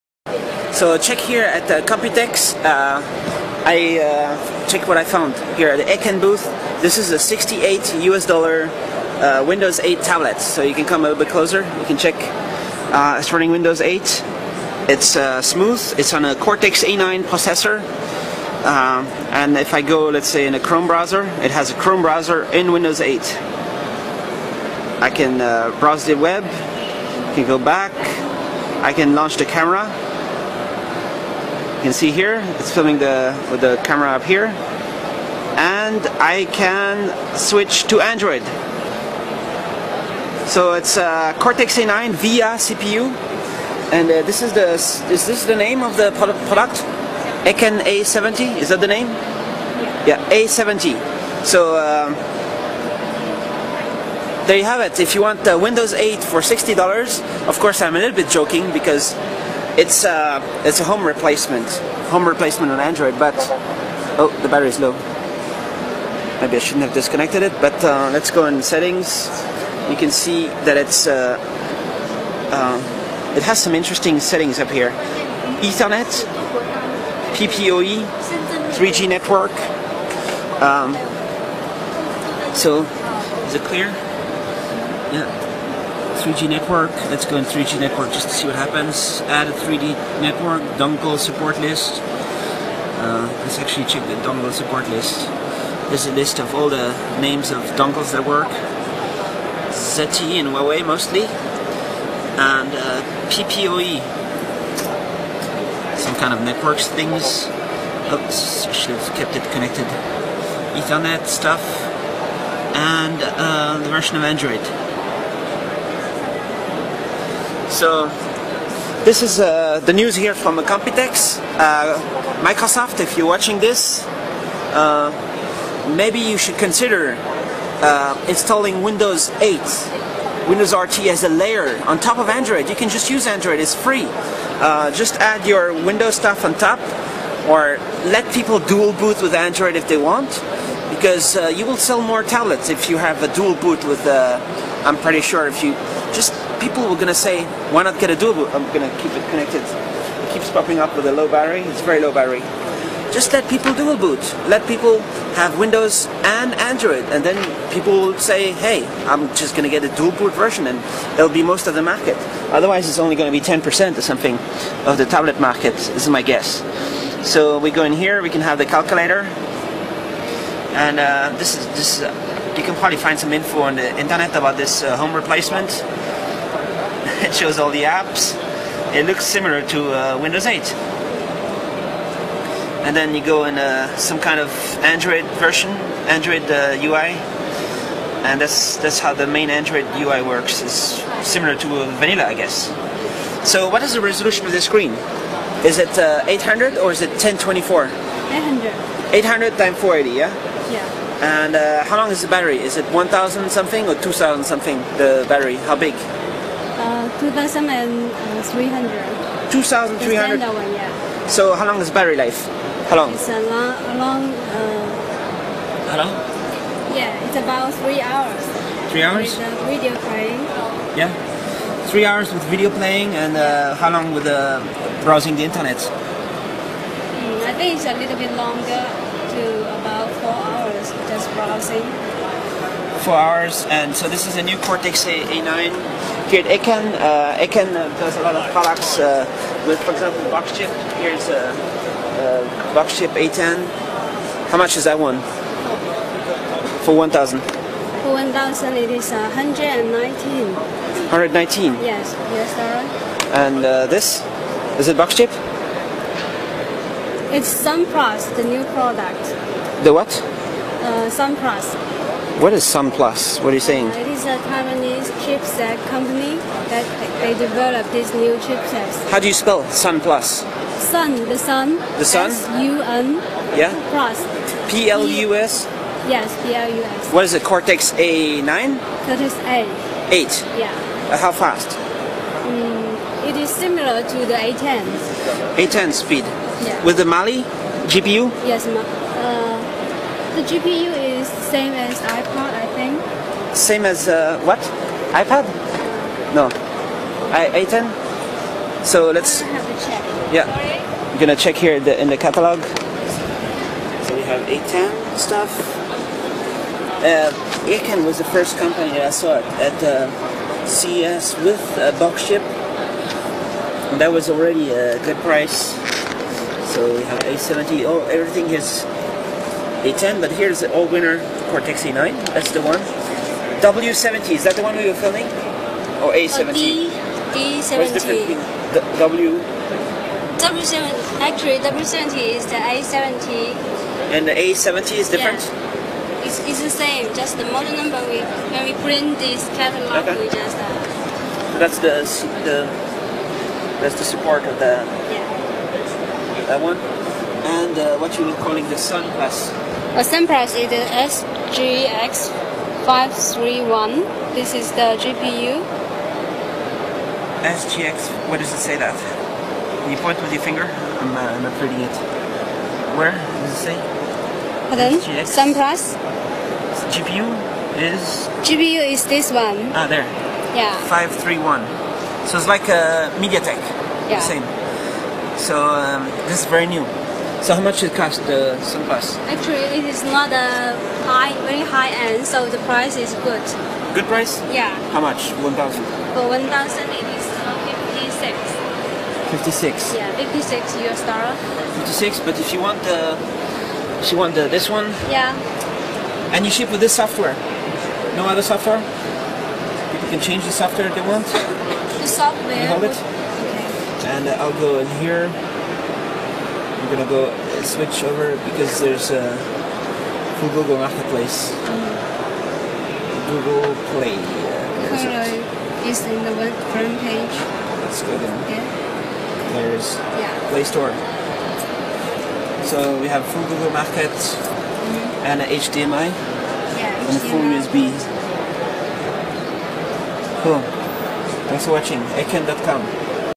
Windows 8 On Eken A70 Android Tablet
Eken A70 installed Android 4.0.3 OS and with customized Windows 8 RT interface.
Main specs: - 7 inch capacitive screen, 1.5GHz powerful ARM cortex A9 processor, 512MB DDR3 RAM, 4GB ROM, 1.3MP front Camera, Android ICS OS ( Android 4.0.3)